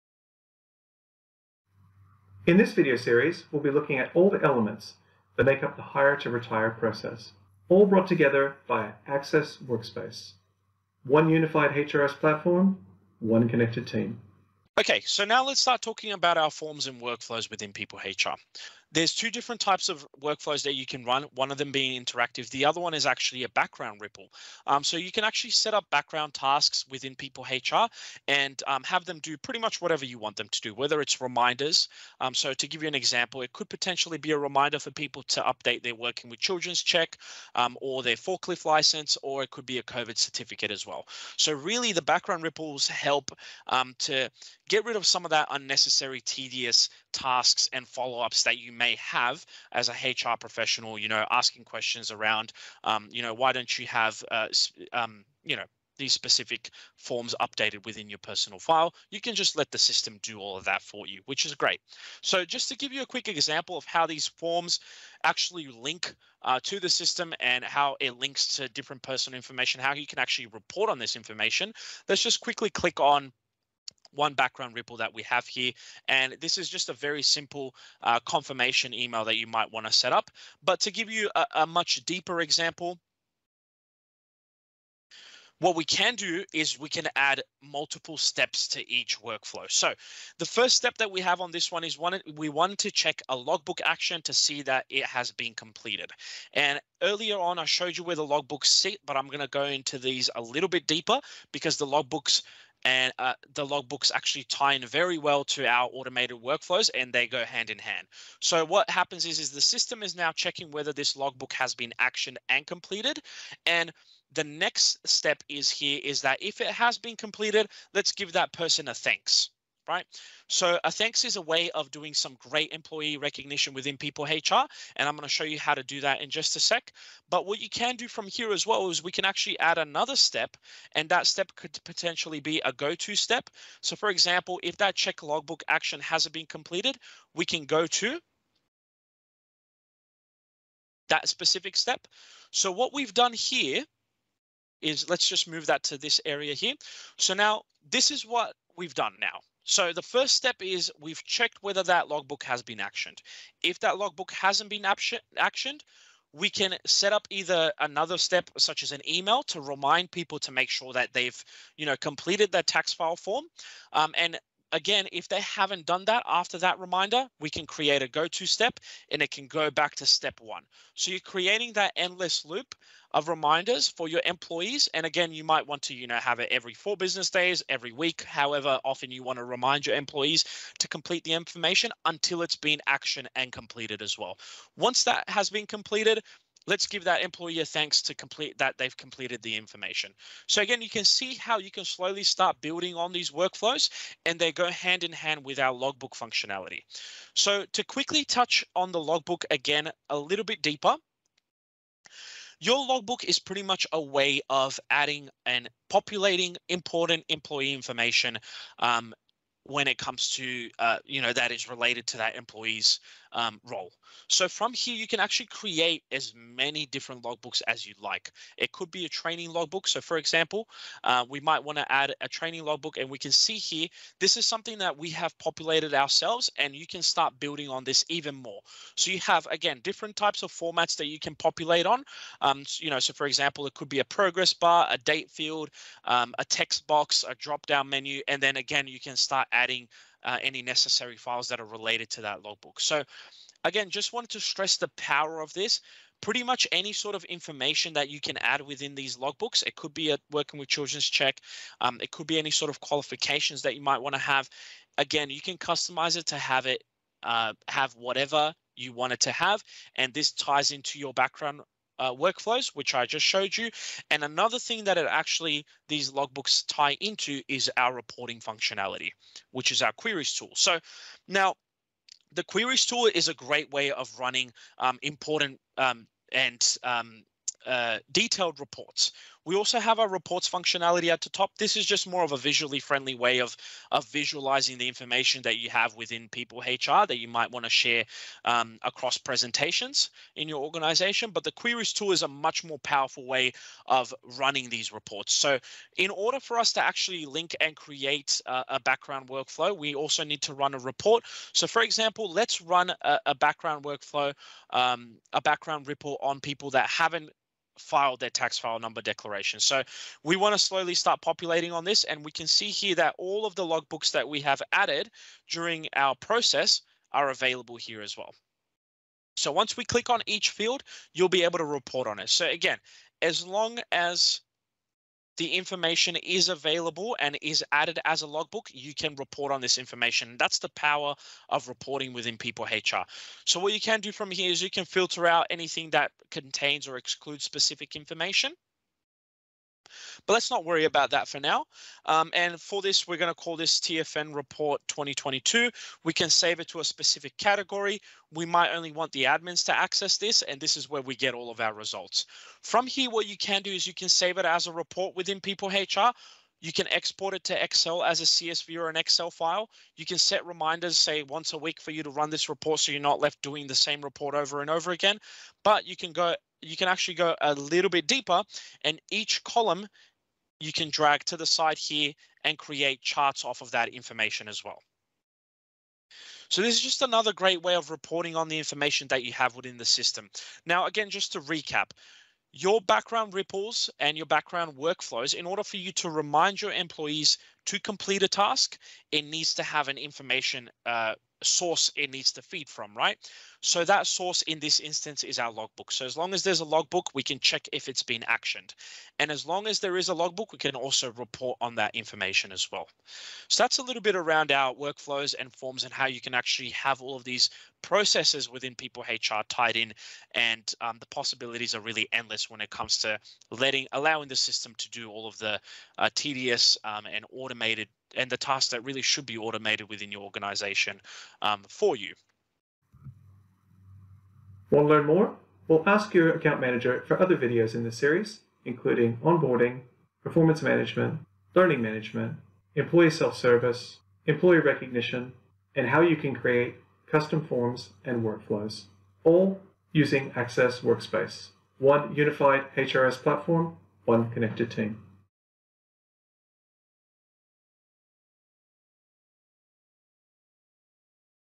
Access PeopleHR – Forms Workflows Reporting
Ripple – the workflow automation engines helps you to remove unnecessary and tedious tasks or follow ups with employees. You will be able to update task and run queries to build a specific report that you need.